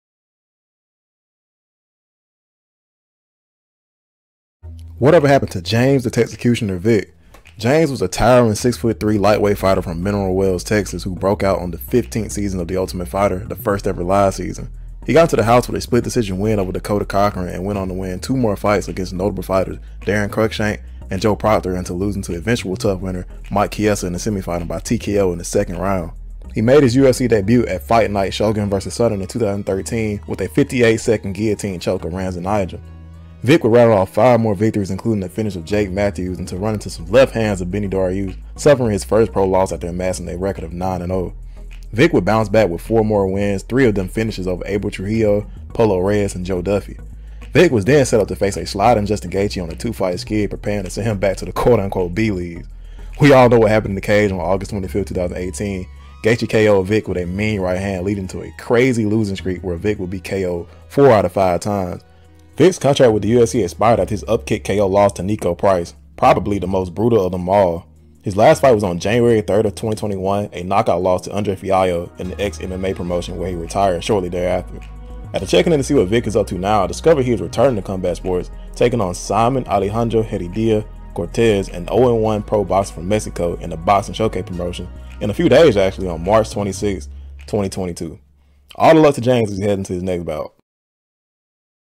What Happened To UFC Lightweight James Vick?
Yooo welcome to WhatHappenedMMA the channel where I look back at some more recent mixed martial artists that walked away, went to less prestigious promotions, or flamed out of the sport completely.